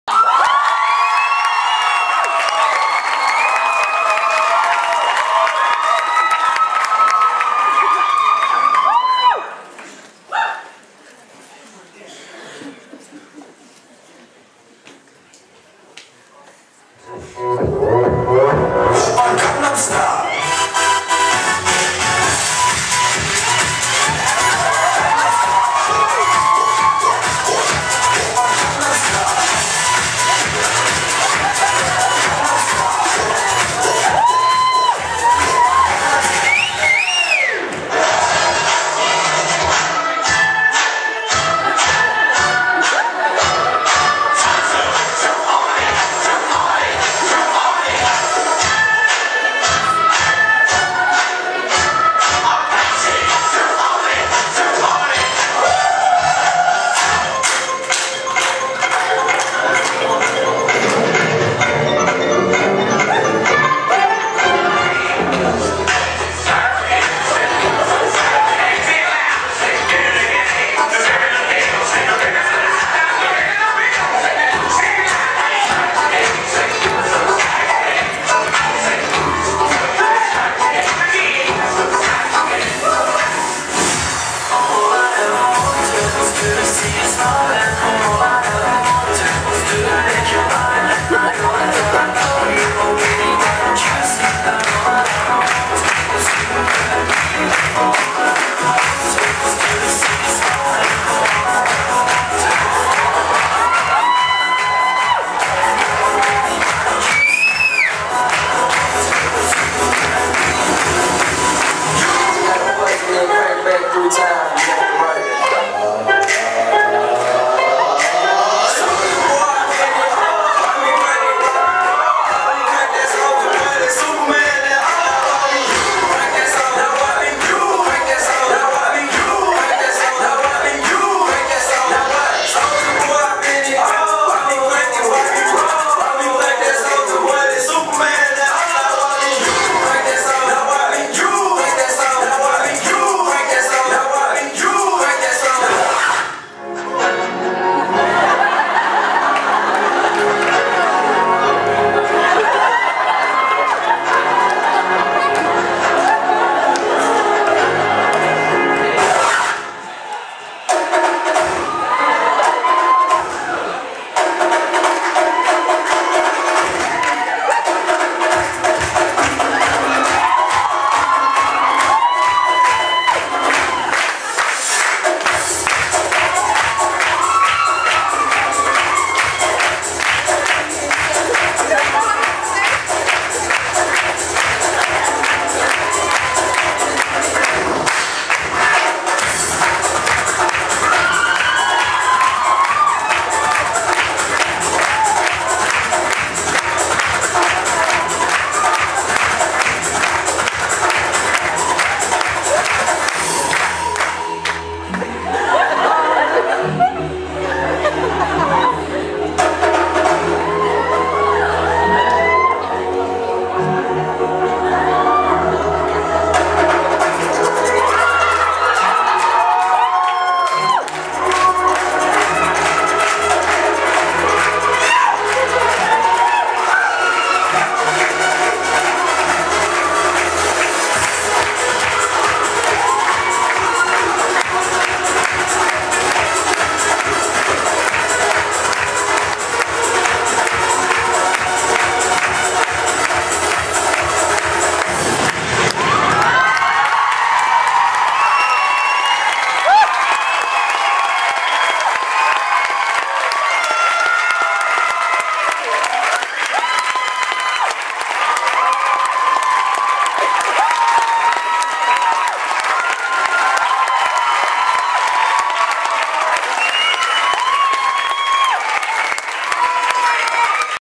Dawnsio Disgo Bois 2012
Carwyn, Chunk, Alun, James, Chris, Steff, Nathan a Tomos yn ennill y cystadleuaeth Dawnsio Disgo yn Eisteddfod CFfI Sir Gâr 2012